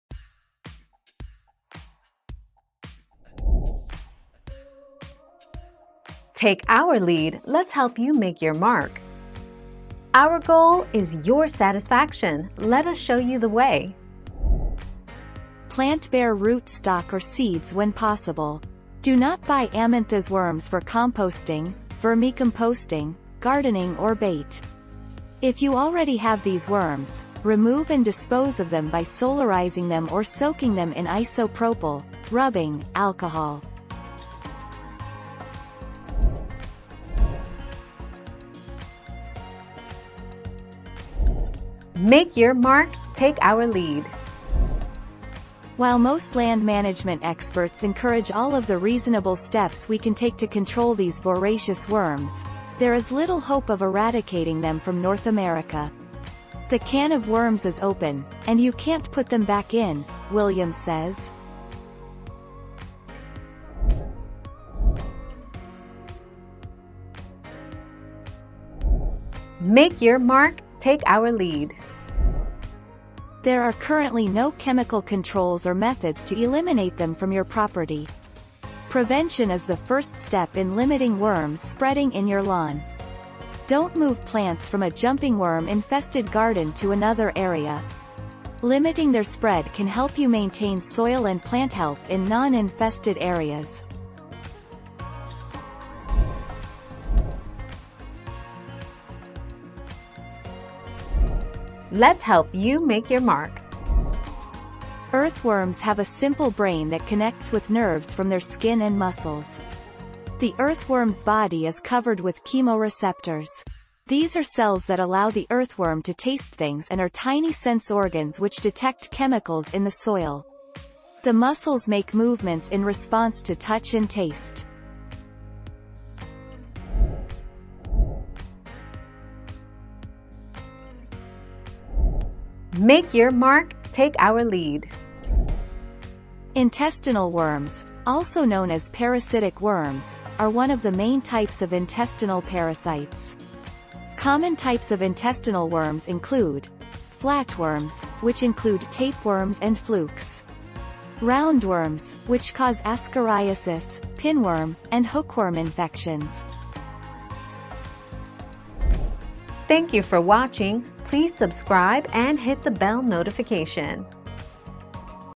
What do you do if you have jumping worms?
00:00 - What do you do if you have jumping worms?
00:38 - Should you kill jumping worms?
01:05 - Can you get rid of jumping worms?
01:37 - Why do worms wiggle when you touch them?
02:08 - Can worms live in the human stomach?

Laura S. Harris (2021, July 9.) What do you do if you have jumping worms?
    AskAbout.video/articles/What-do-you-do-if-you-have-jumping-worms-257654